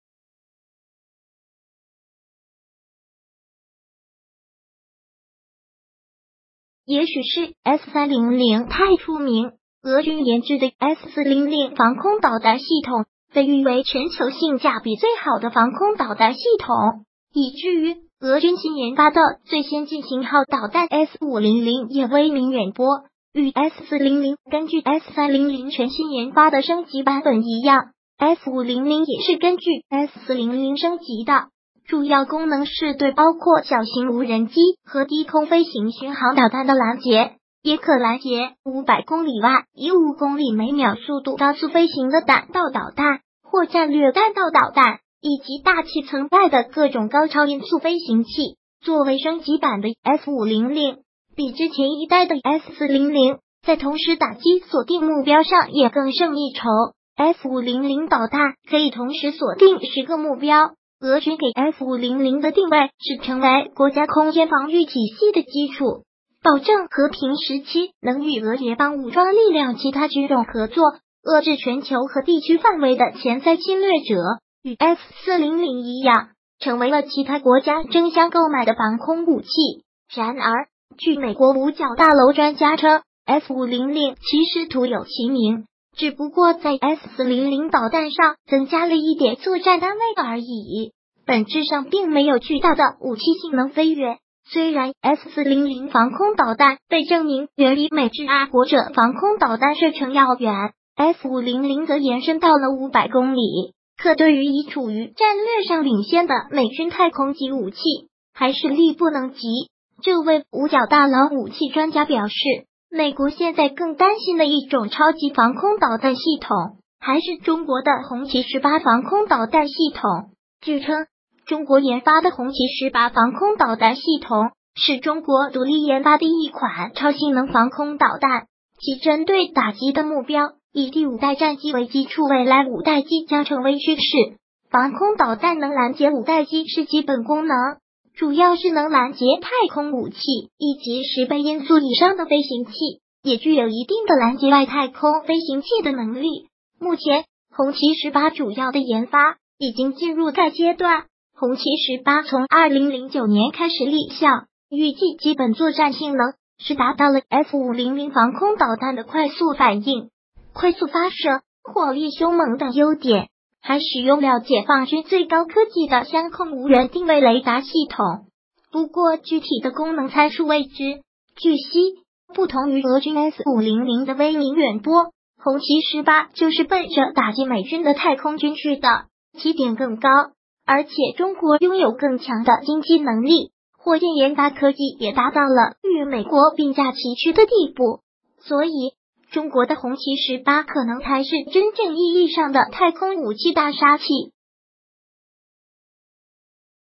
俄军S500最强防空导弹？五角大楼专家：红旗18才是太空武器大杀器
也许是S-300太出名，俄军研制的S-400防空导弹系统被誉为全球性价比最好的防空导弹系统，以至于俄军新研发的最先进型号导弹S-500也威名远播！与S-400根据S-300全新研发的升级版本一样，S-500也是根据S-400升级的，主要功能是对包括小型无人机和低空飞行巡航导弹的拦截，也可拦截500公里外以5公里/秒速度高速飞行的弹道导弹或战略弹道导弹，以及大气层外的各种高超音速飞行器。作为升级版的S-500，比之前一代的S-400在同时打击锁定目标上也更胜一筹，S-500导弹可以同时锁定10个目标！俄军给S-500的定位是成为国家空天防御体系的基础，保证和平时期能与俄联邦武装力量其他军种合作，遏制全球和地区范围的潜在侵略者！与S-400一样，成为了其他国家争相购买的防空武器。然而，据美国五角大楼专家称S-500其实“徒有其名”，只不过在S-400导弹上增加了一点作战单位而已，本质上并没有巨大的武器性能飞越。虽然S-400防空导弹被证明远比美制爱国者防空导弹射程要远，S-500则延伸到了500公里，可对于已处于战略上领先的美军太空级武器还是力不能及。这位五角大楼武器专家表示，美国现在更担心的一种超级防空导弹系统还是中国的红旗18防空导弹系统。据称，中国研发的红旗-18防空导弹系统是中国独立研发的一款超性能防空导弹，其针对打击的目标以第五代战机为基础（未来五代机将成为趋势，防空导弹能拦截五代机是基本功能），主要是能拦截太空武器以及10倍音速以上的飞行器，也具有一定的拦截外太空飞行器的能力！目前，红旗-18主要的研发已经进入该阶段。红旗-18从2009年开始立项，预计基本作战性能是达到S-500防空导弹的快速反应，快速发射、火力凶猛等优点，还使用了解放军最高科技的相控无源定位雷达系统，不过具体的功能参数未知！据悉，不同于俄军S-500的威名远播，红旗-18就是奔着打击美军的太空军去的，起点更高，而且中国拥有更强的经济能力，火箭研发科技也达到了与美国并驾齐驱的地步，所以，中国的红旗-18可能才是真正意义上的太空武器大杀器。